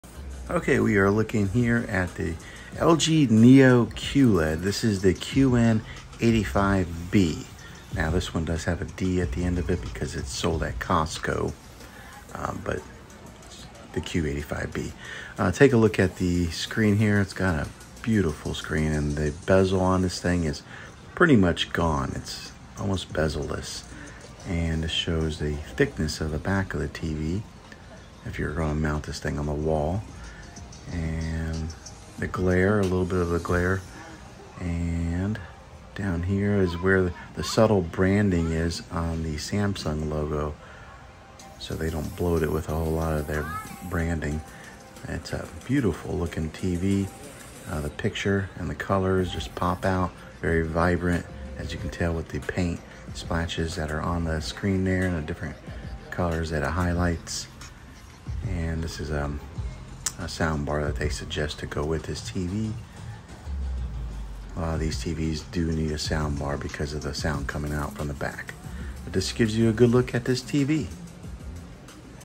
Samsung QN85B/QN85BD OLED TV - Detailed Look
Here is a close up view of the Samsung QN85B, also known as the QN85BD OLED TV.